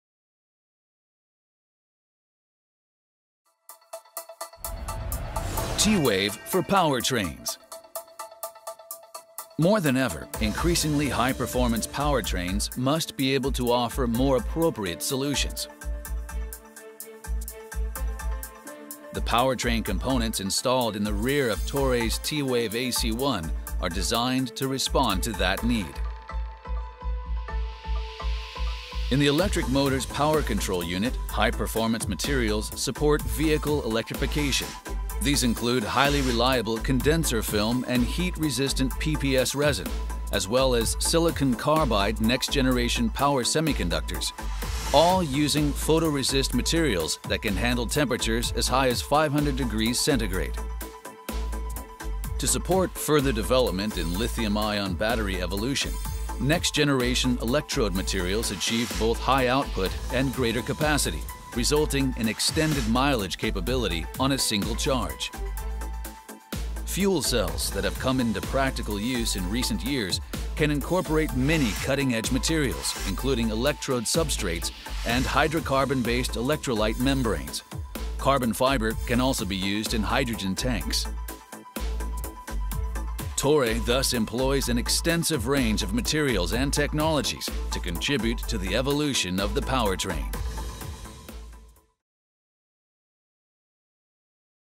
【TORAY】TEEWAVE® for Powertrains
Toray has decades of experience in developing materials and components for automobile powertrain systems.
With products ranging from lightweight, heat-resistant engineering plastics for internal combustion engine parts, to electronic devices for controlling electric vehicle motors, and PPS polymers and films for batteries and digital information processing systems, Toray is driving the future of the automobile industry.